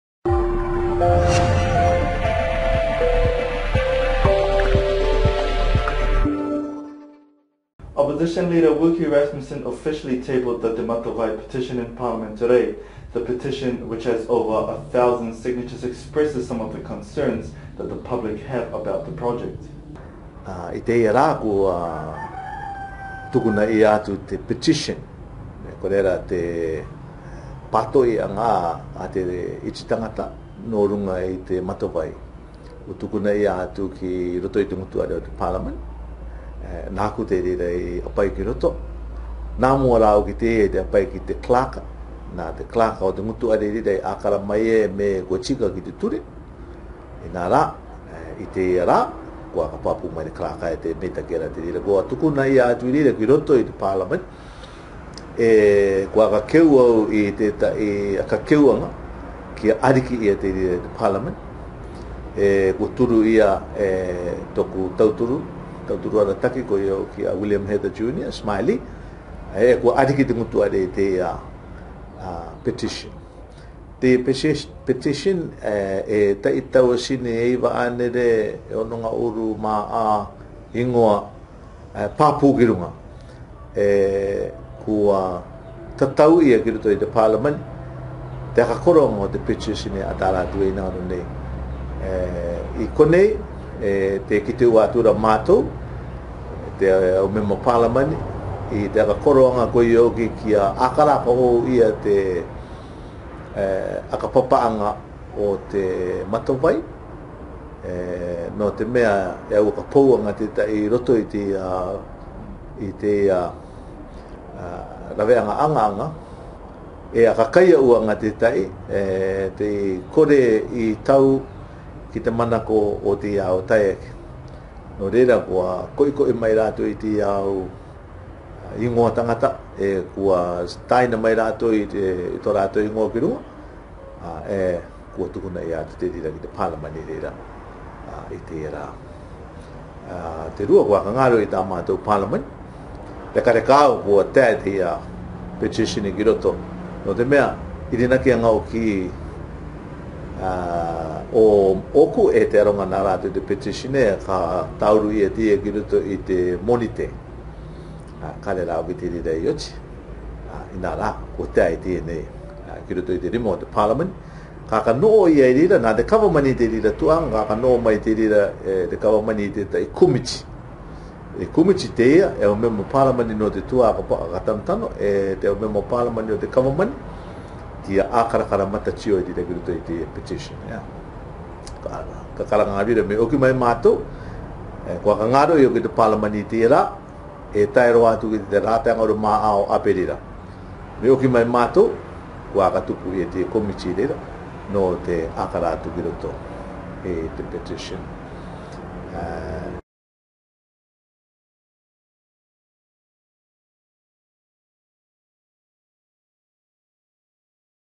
Te Mato Vai Petition
The contriversal  " TM Vai  and TM Heather Conflict of Interest"  the largest project undertaken and  co funded by China, NZ and the CIP Government.

This petition signed by 1964 Cook Islanders  was presented to parliament by the Demo Opposition to oppose the outrageous expense $60 million plus...and the  conflict concerns of Minister of Infrastructure  Teariki Heather involvement and his company TM Heather.